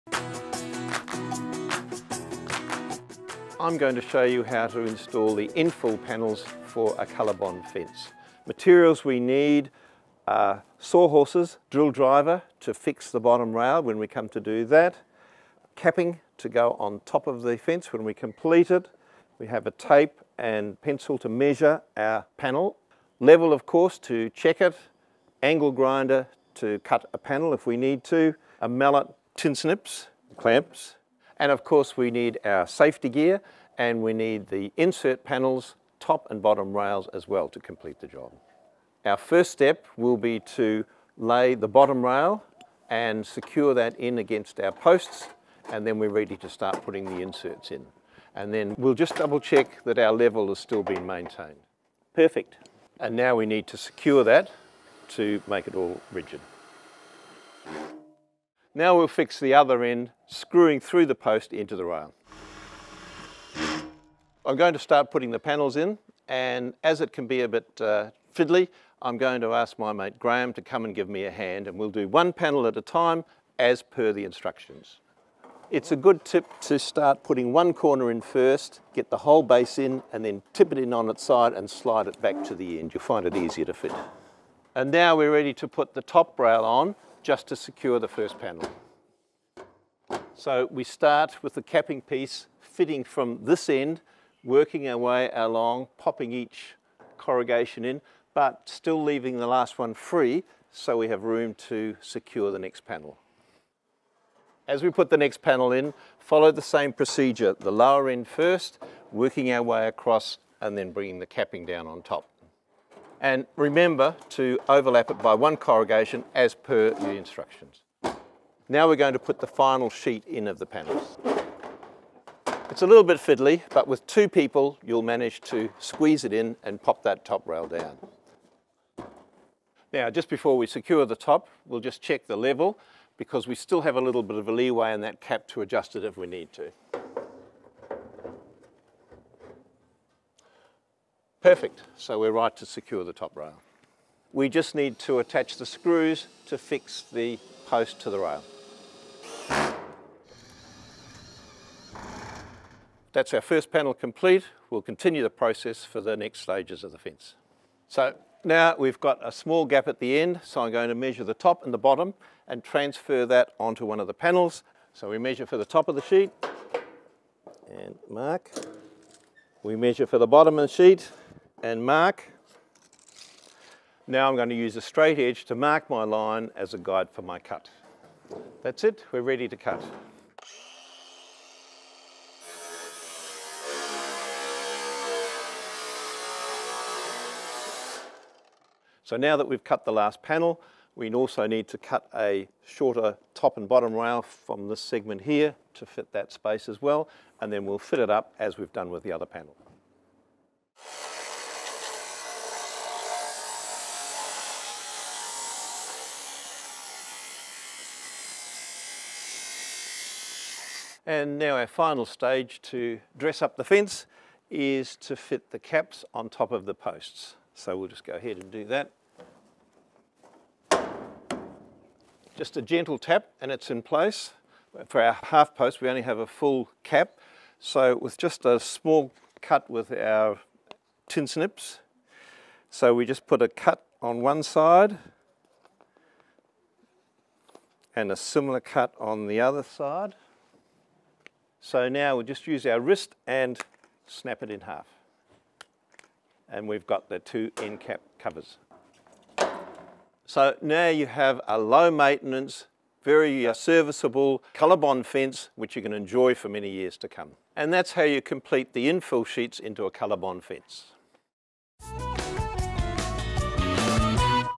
How To Install Colorbond Fence Panels - DIY At Bunnings
Installing the infill sheets for a Colorbond fence is relatively easy but you will need someone to help you. This step-by-step video guide from Bunnings Warehouse shows you how to install and secure the top and bottom rails, install the infill sheets, along with how to cut the sheets and posting caps to size.